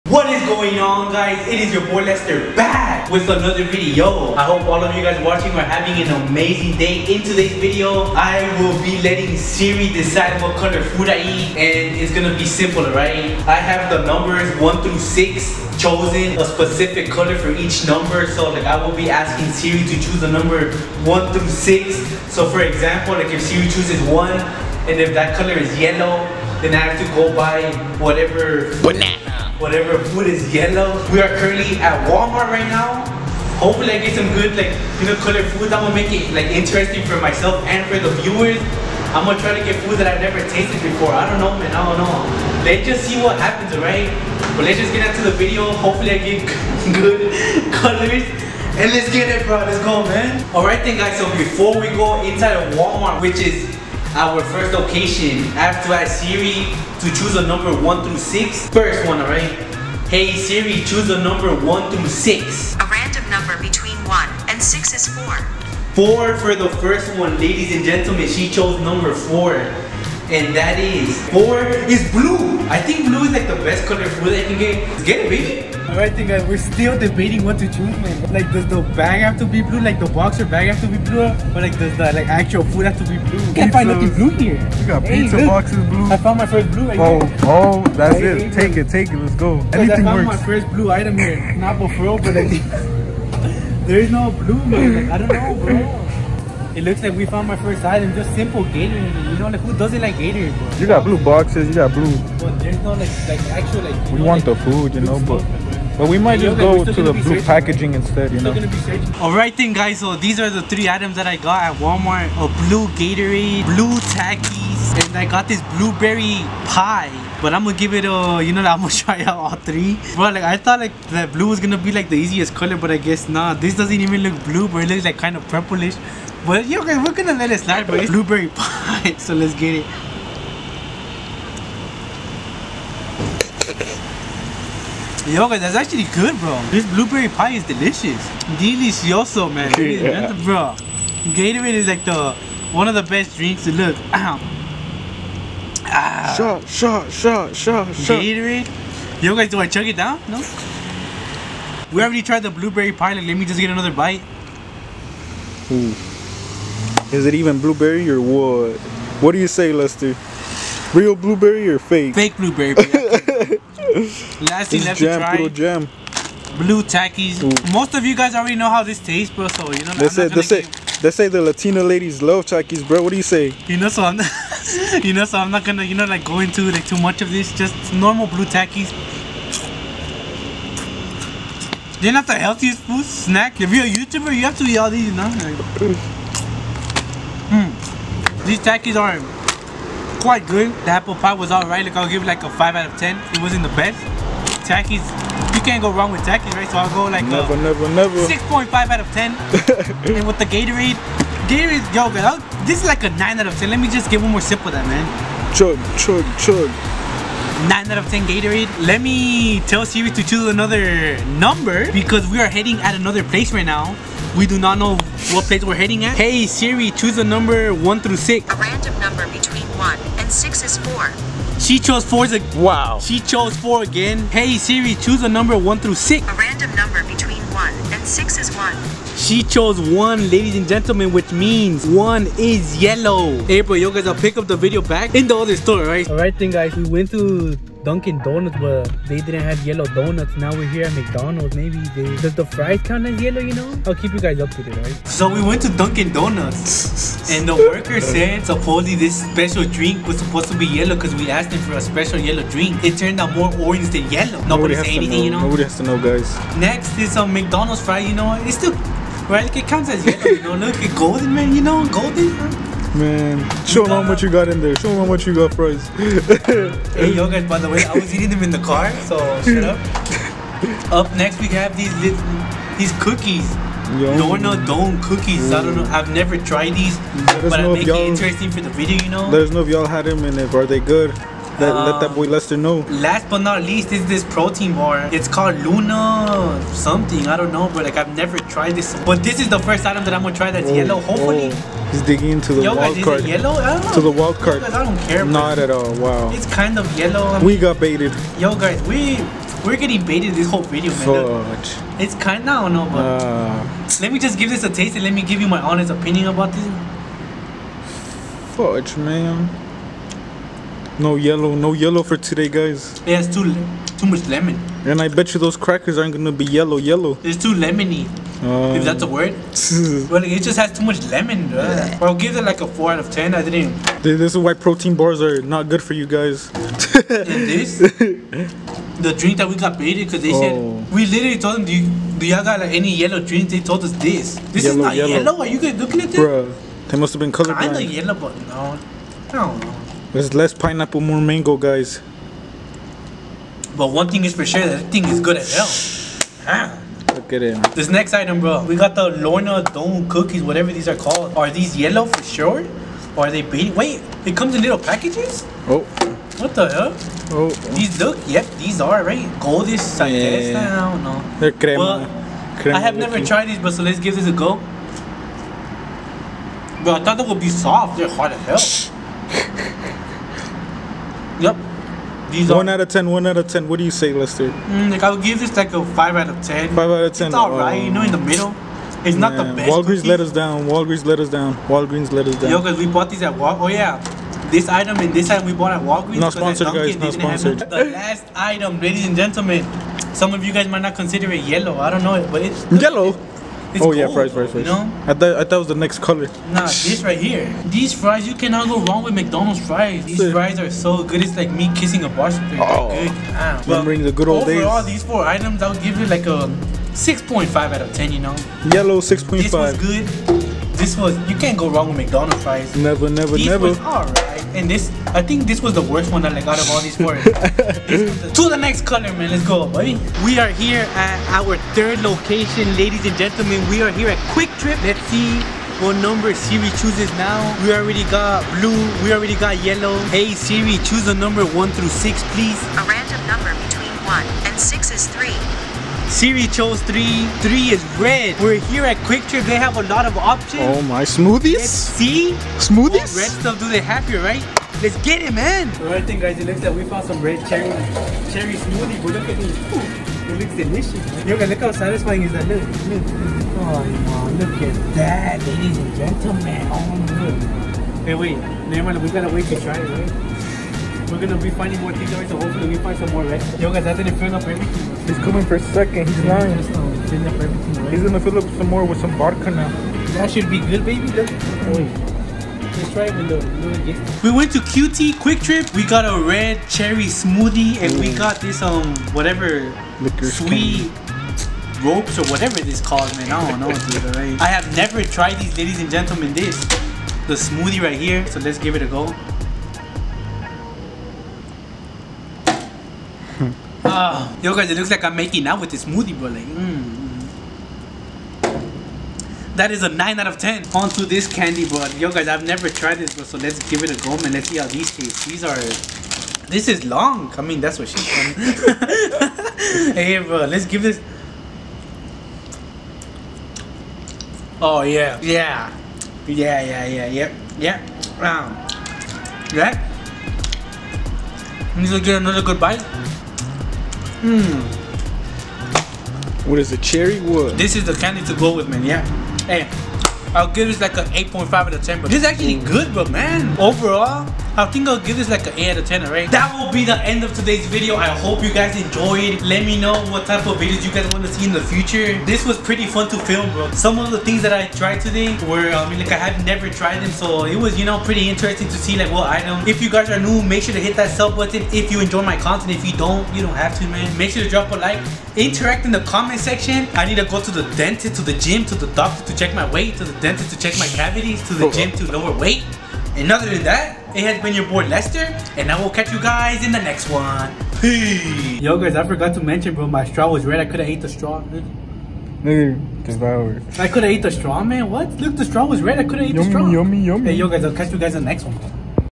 I Let SIRI DECIDE What Color Food I Eat!!
I will be letting siri decide what color food I eat. This was a very fun challenge I hope you guys enjoy.